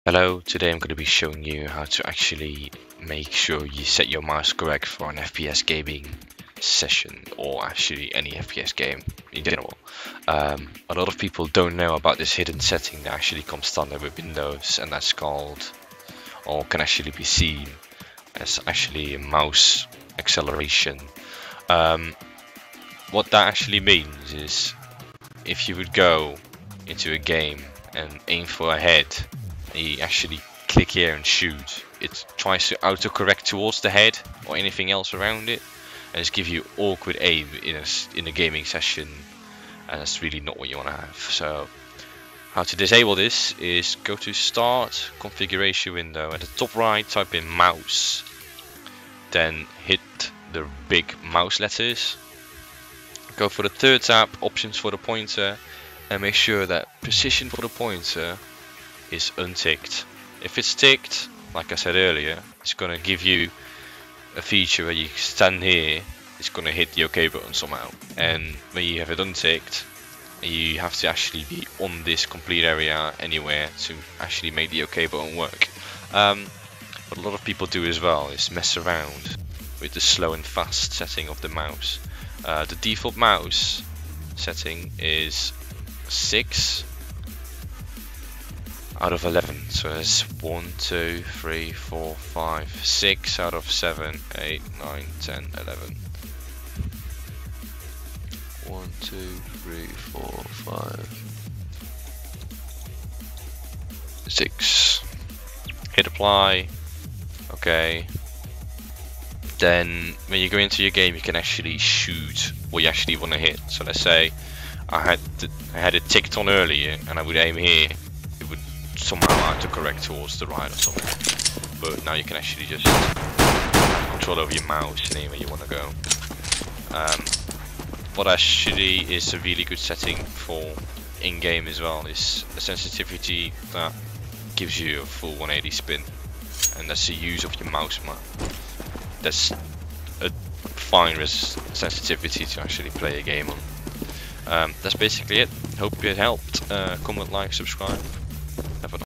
correct mouse settings for FPS Games such as CS:GO and CS:S
How to set up your mouse correct for any FPS game.

In game settings:
Config:

A like would help but only press LIKE if you really LIKE'D the video ofcourse.comments are welcome aswell :D so are subscribers to my youtube channel.